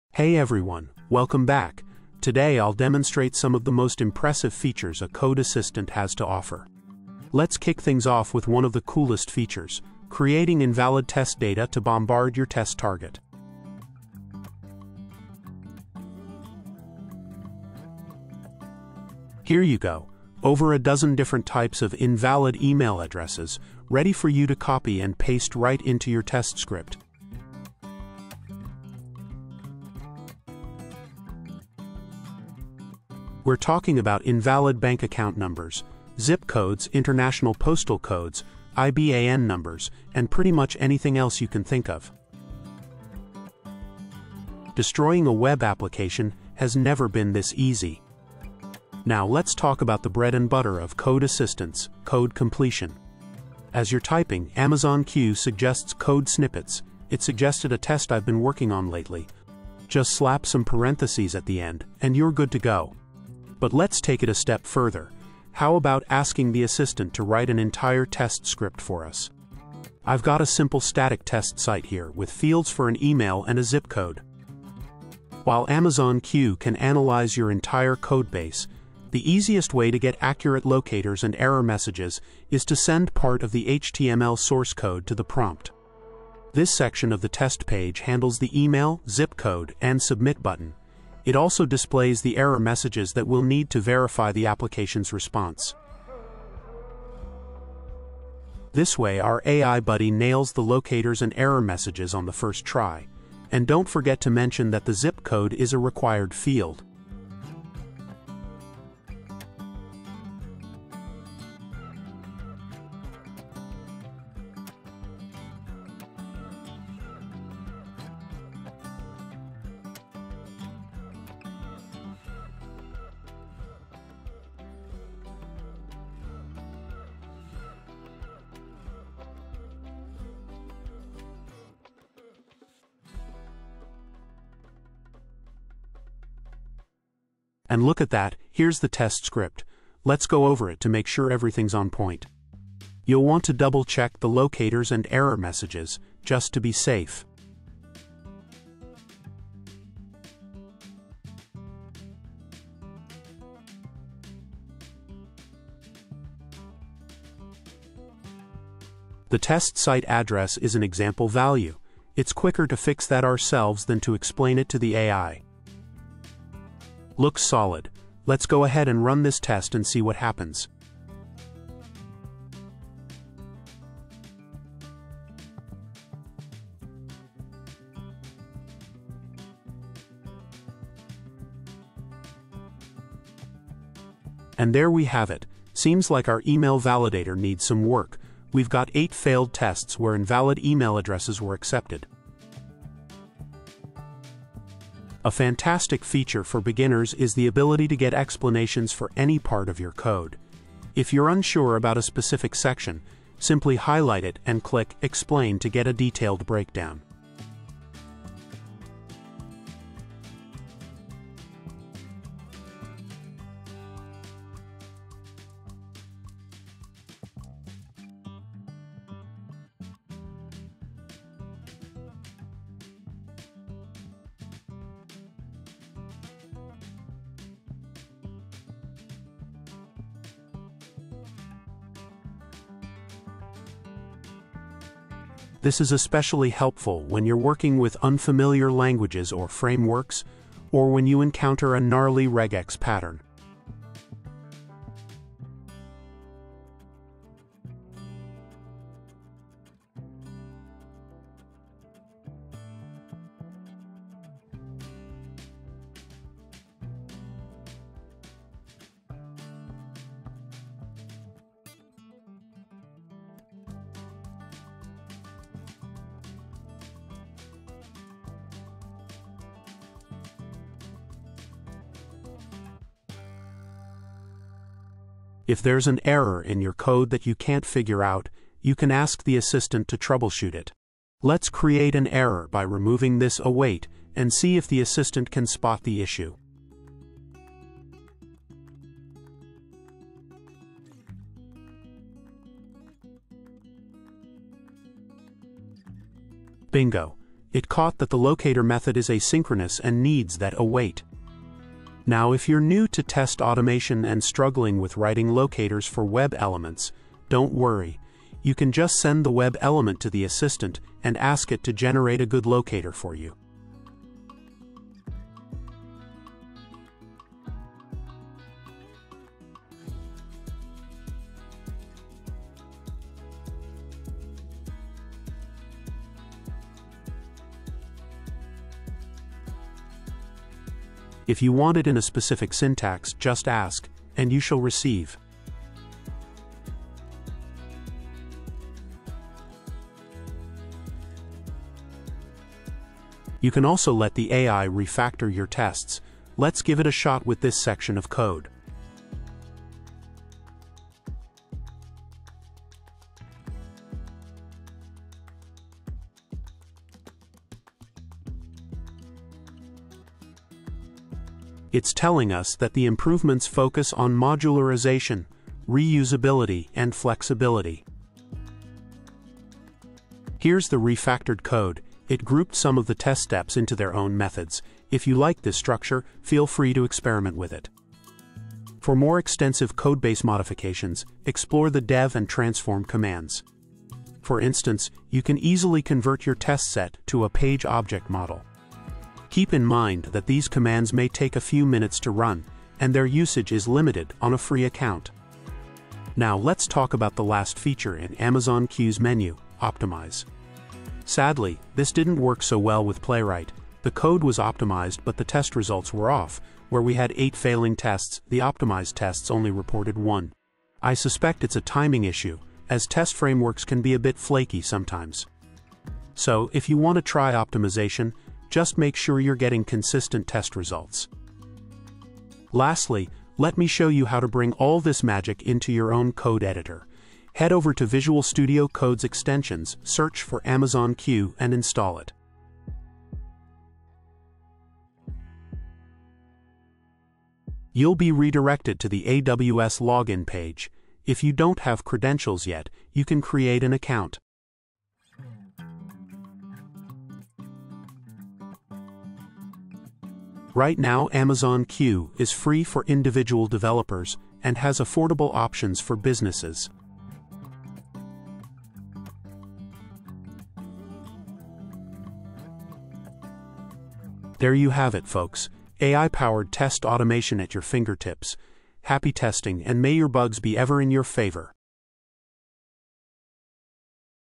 Amazon Q for Playwright Test Automation
Today I'll demonstrate some of the most impressive features a code assistant has to offer.

Chapters
00:00 - Creating Test Data & Tests with Amazon Q
3:14 - Explain with Amazon Q
4:34 - Fixing Errors with Amazon Q
5:43 - Refactoring with Amazon Q
7:02 - Installing Amazon Q for Developers Extension